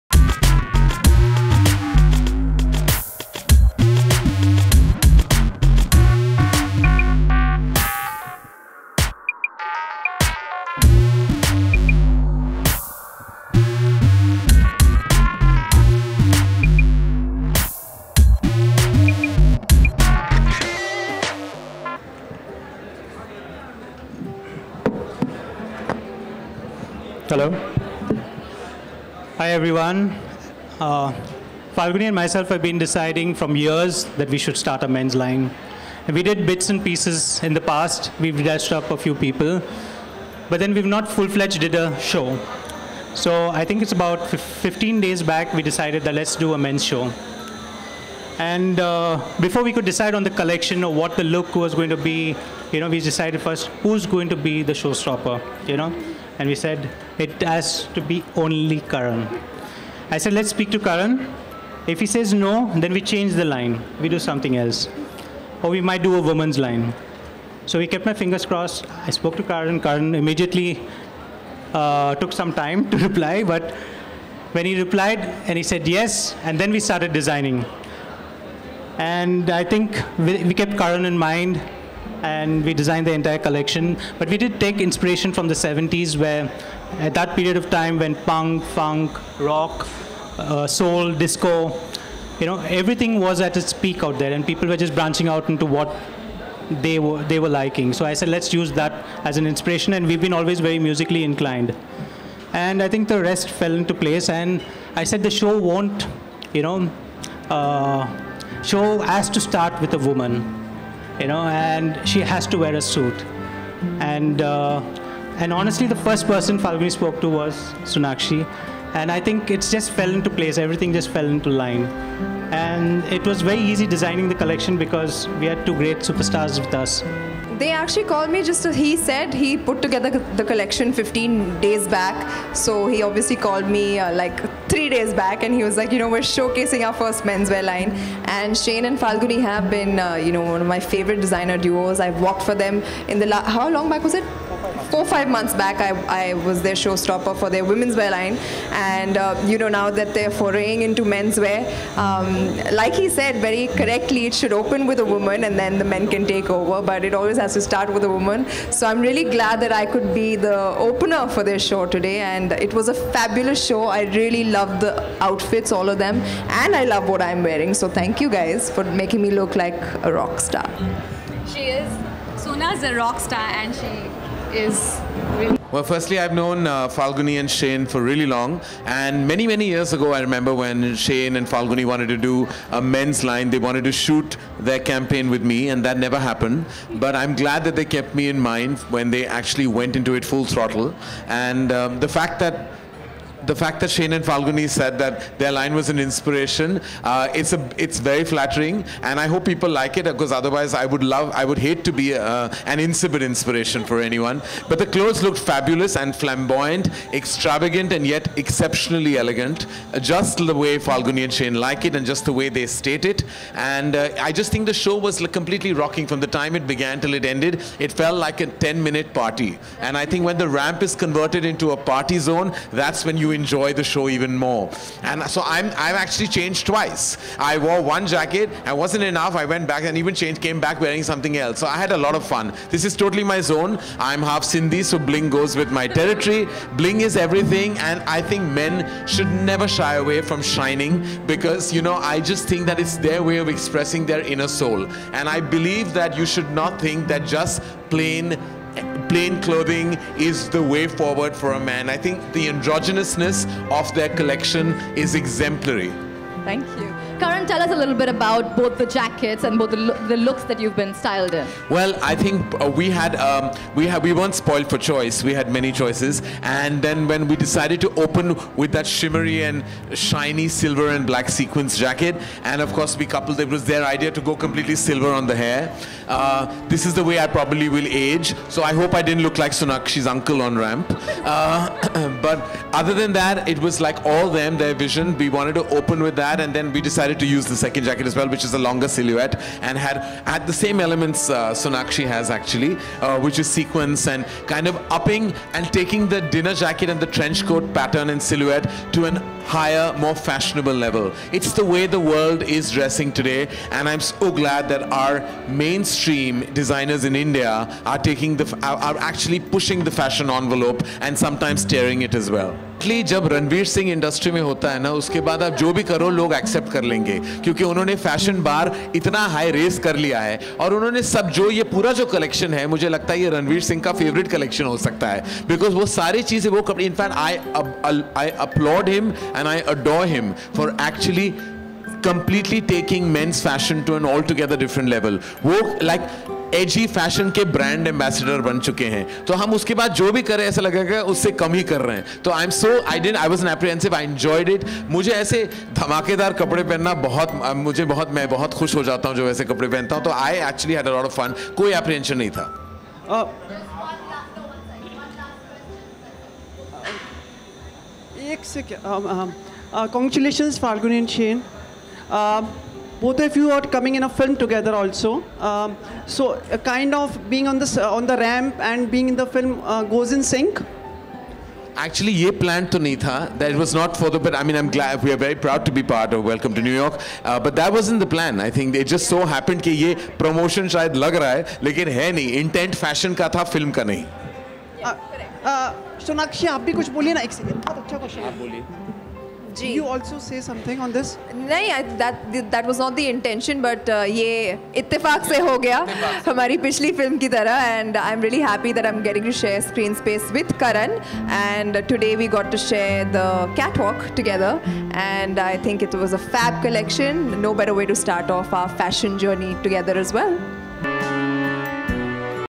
Sonakshi Sinha & Karan Johar FULL INTERVIEW @ LAKME FASHION WEEK ! BWOODS WRAP
FOR MORE UPDATE WATCH VIDEO